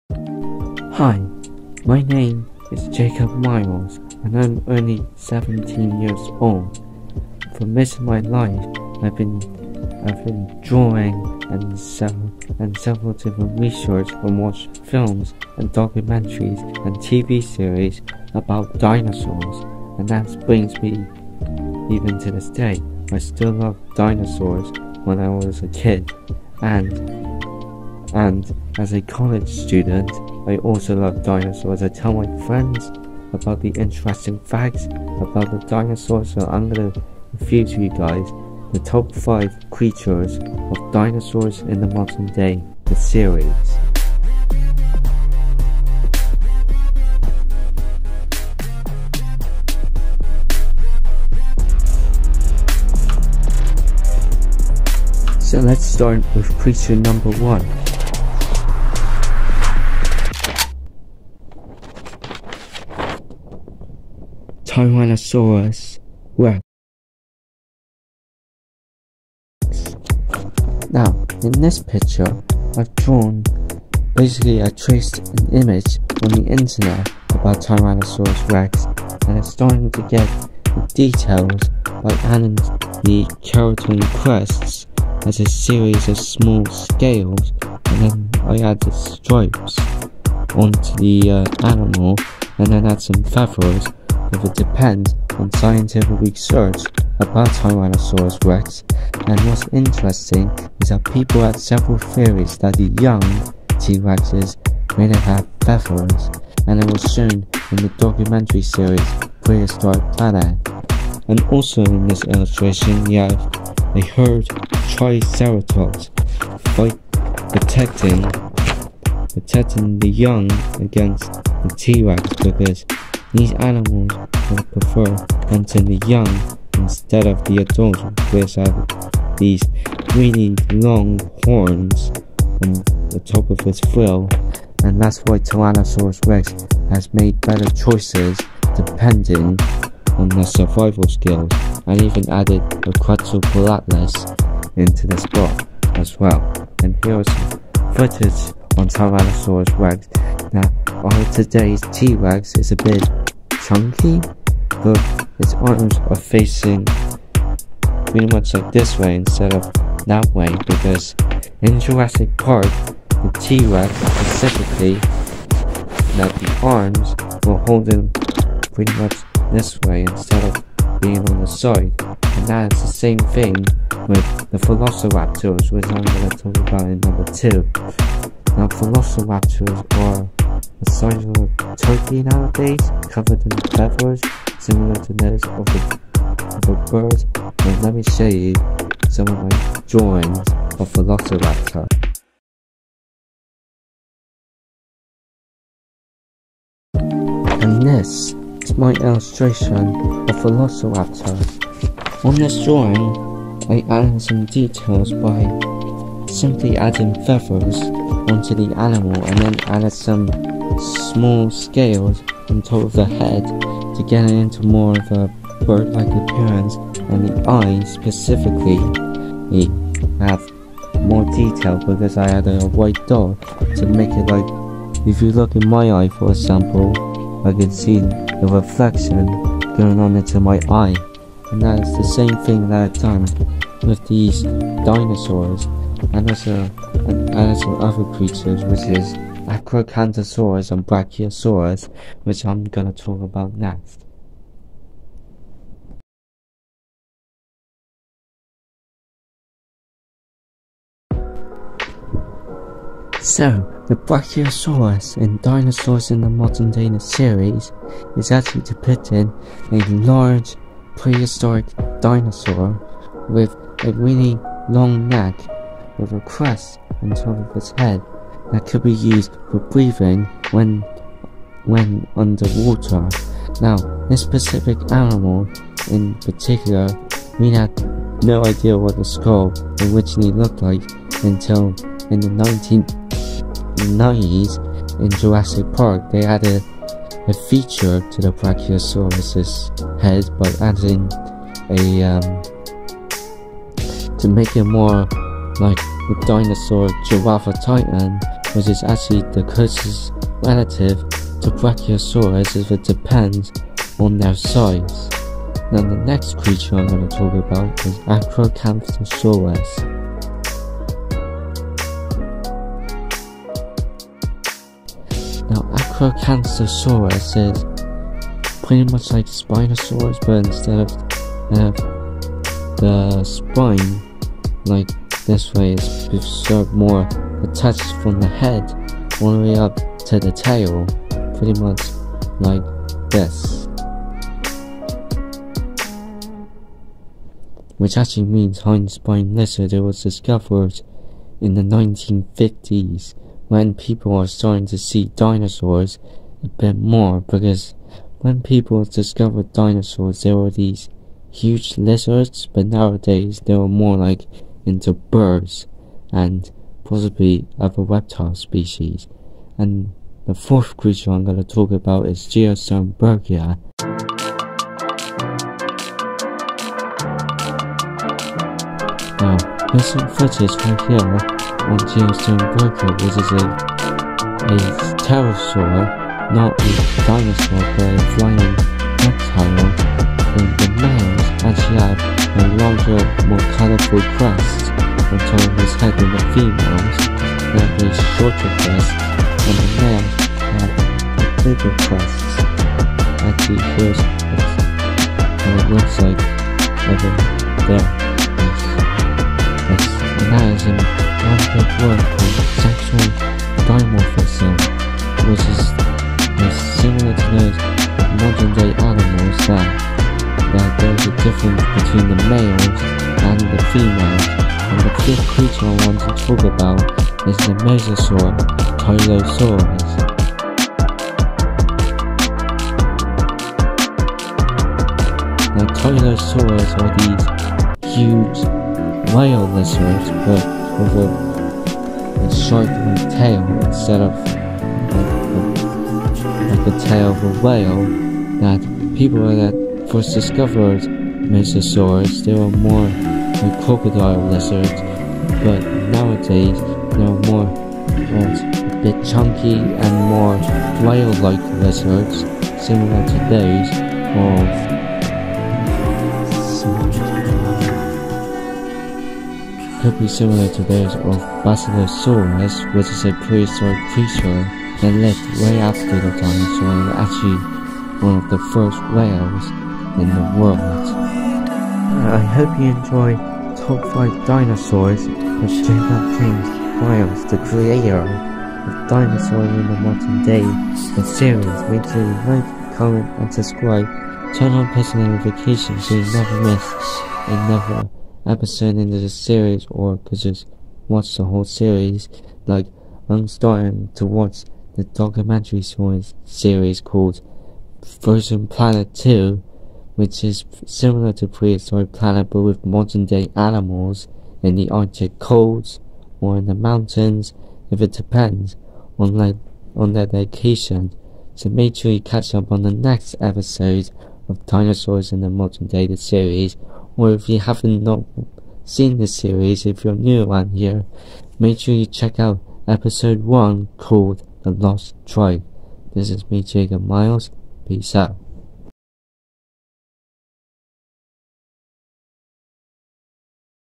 The Top 5 Dinosaurs In Dinosaurs In The Modern Day The Series
This is the 1st top 5 videos on Dinosauria Studios were this video it is about the top 5 creatures edited on the windows version of CapCut video editor on the YouTube Documentary Series Dinosaurs In The Modern Day The Series.